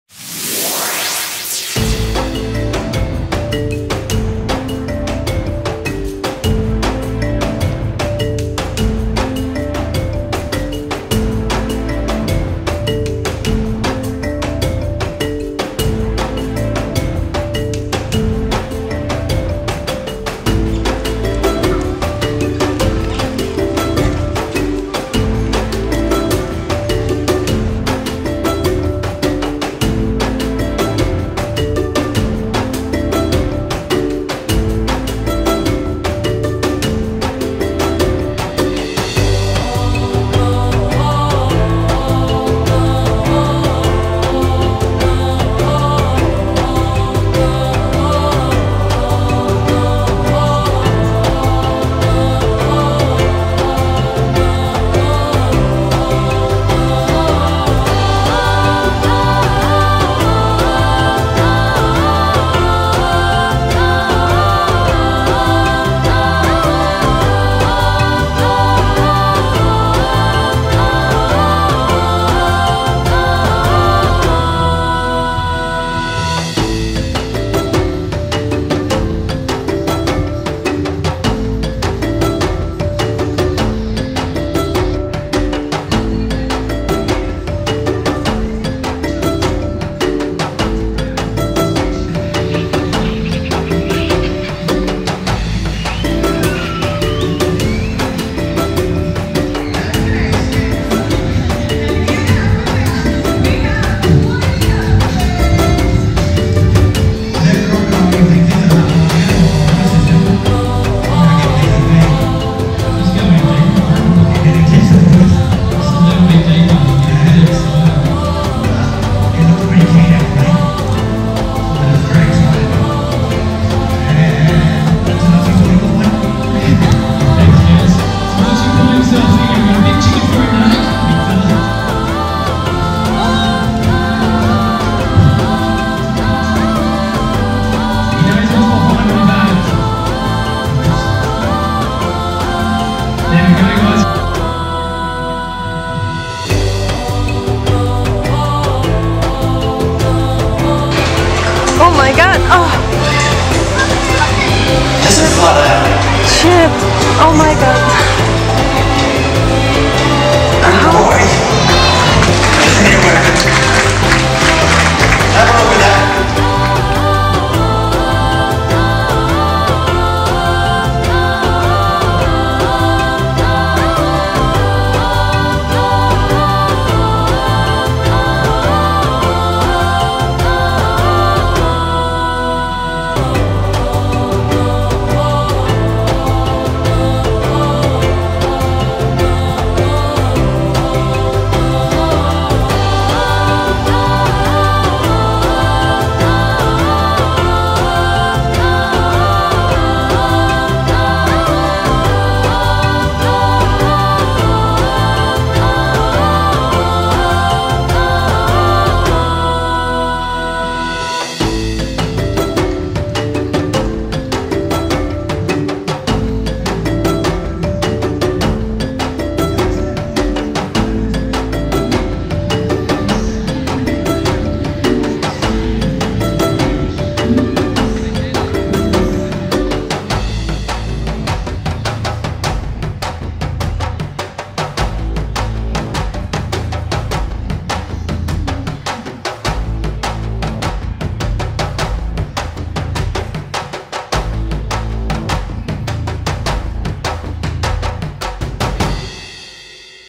Australia Zoo Crocoseum: Crocodile Show with Wildlife Warriors
The World-Famous Australia Zoo Crocoseum is the fulfillment of one of Steve Irwin's lifelong dreams.  Their keepers and their amazing animals will take us on a slithering, swooping, jaw-snapping adventure of epic proportions their army of Wildlife Warriors! Woo-hoo!

📹GADGETS WE USED👇 👇 👇
✔️Canon EOS 800D / T7i
✔️GoPro HERO9 Black Camera
✔️ Sandisk Extreme 256GB microSD

Australia zoo tour Australia zoo crocodile show